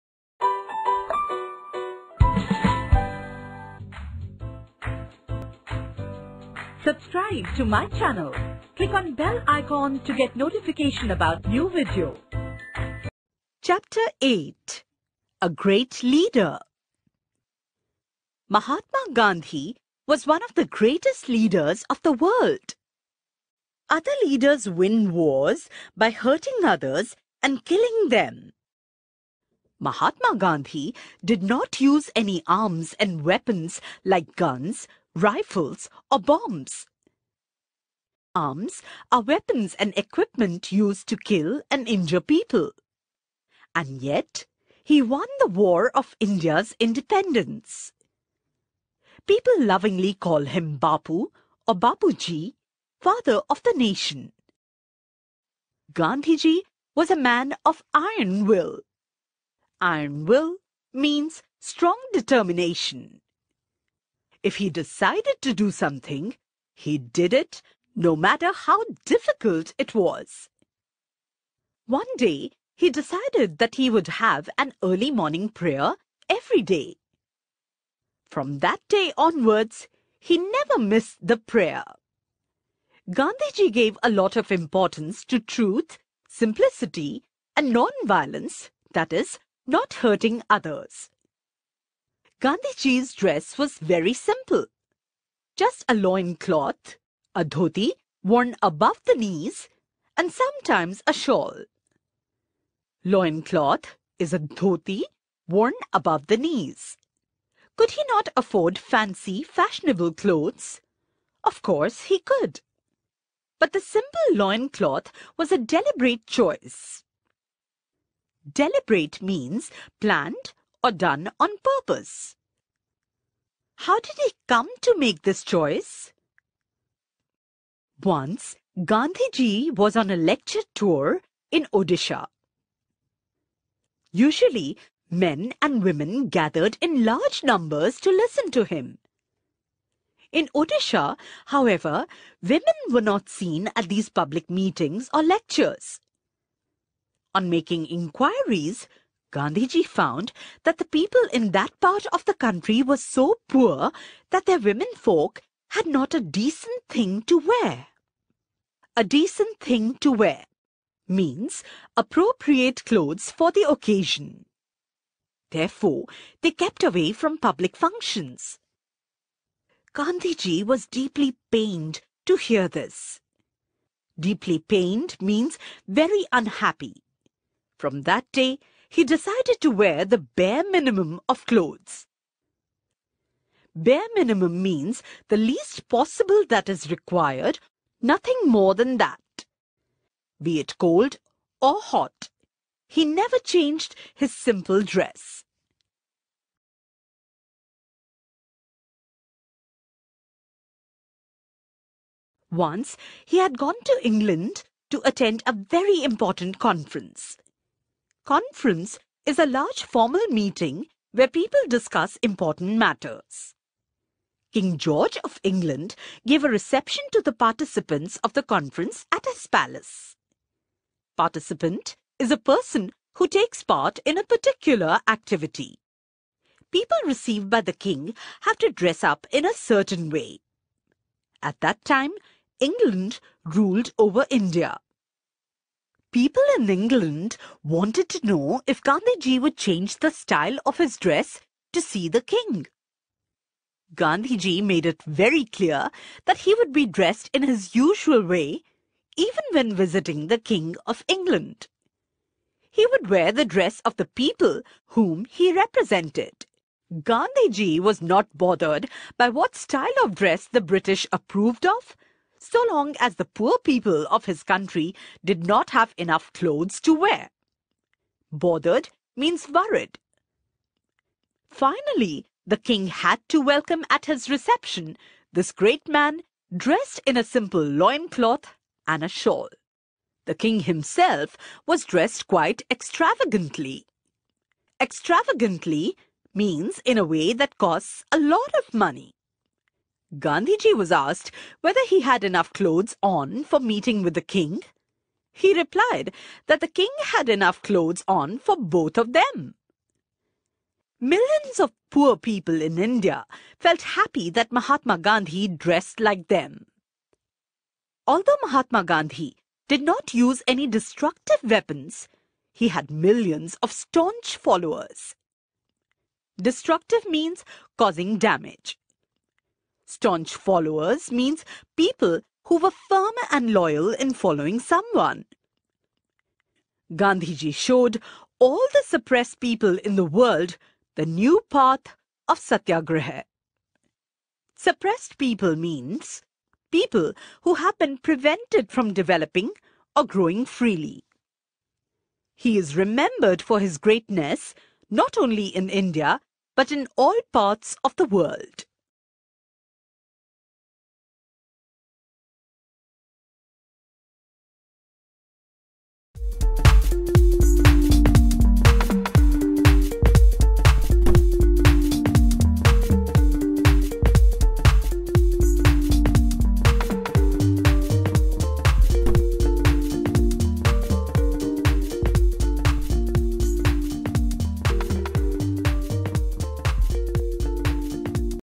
Mahatma Gandhi (महात्मा गांधी) – Great Leader | Class 4 – English | Maharashtra Board | Home Revise
In this video, We have discussed Values, types of Values, Differences between Leader, Administrator, and Reformer.
 First leader Mahatma Gandhi has been discussed in detail.